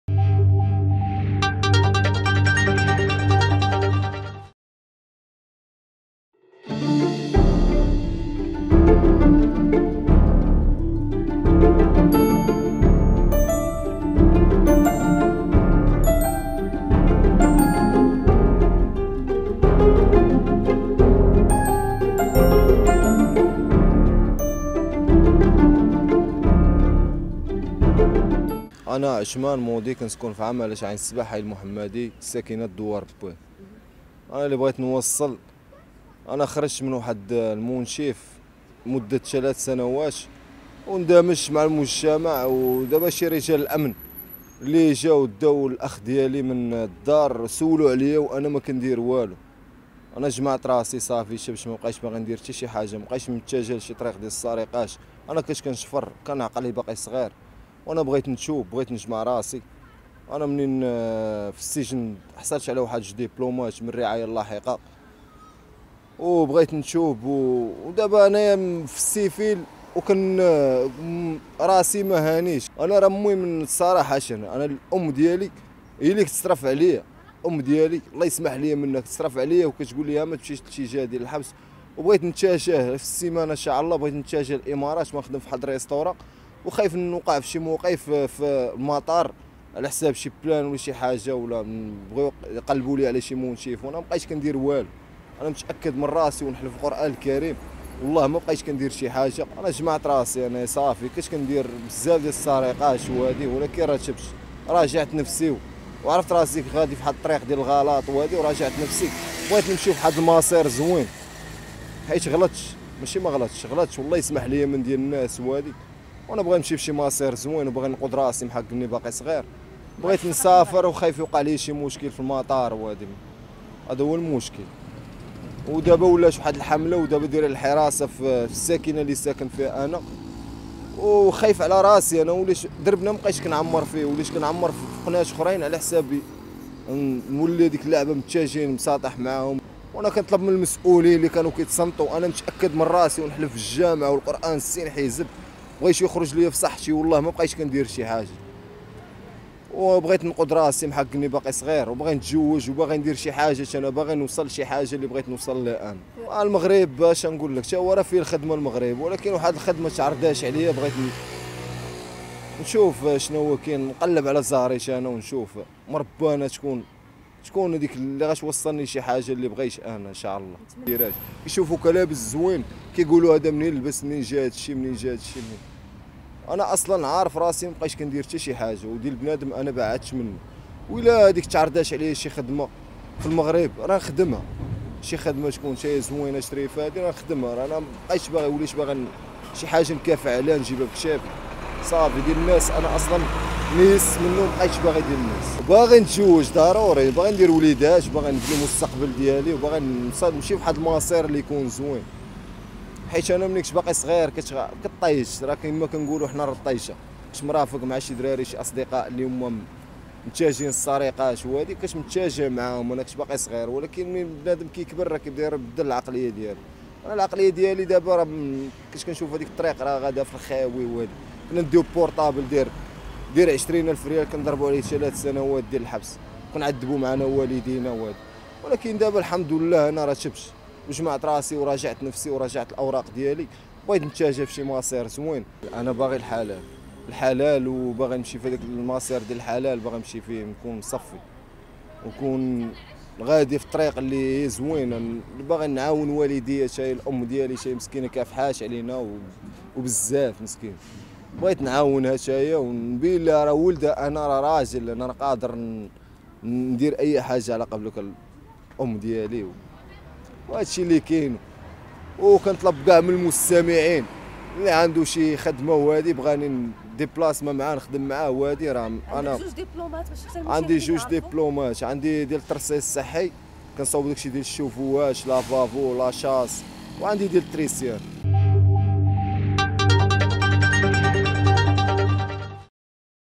سجين سابق بغا يتوب ويتزوج والقصة ديالو جد صادمة:"تشديت على السرقة وبغيت نسافر للإمارات ونخدم"
كشف شاب عن تفاصيل جد مؤثرة خاصة برغبته في إصلاح نفسه عقب قضاء عقوبة سجنية .